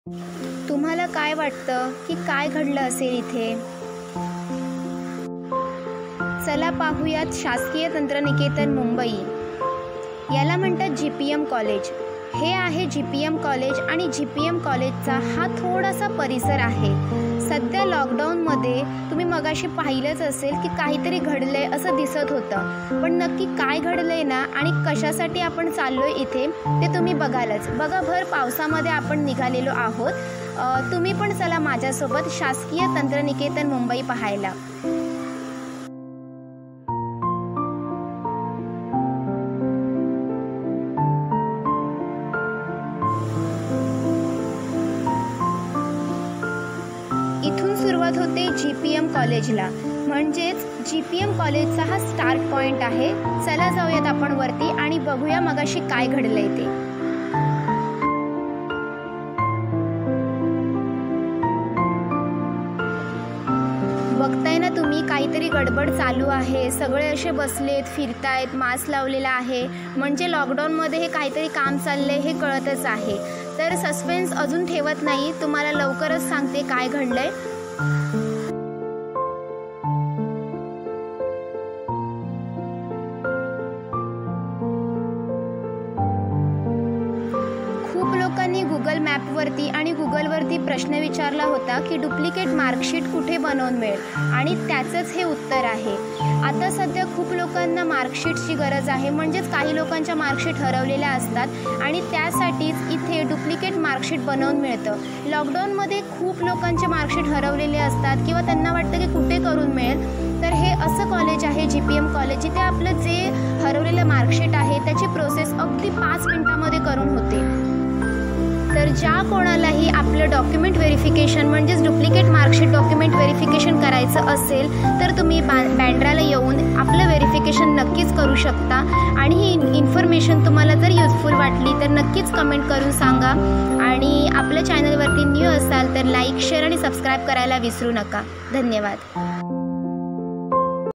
नेमक काय घडलं GPM मध्ये ?GOVERNMENT POLYTECHNIC MUMBAI|POLYTECHNIC#BANDRA#GPM
ALL DOCUMENT VERIFICATION PROCESS AVAILABLE IN GPM COLLEGE DURING LOCKDOWN SO HURRY UP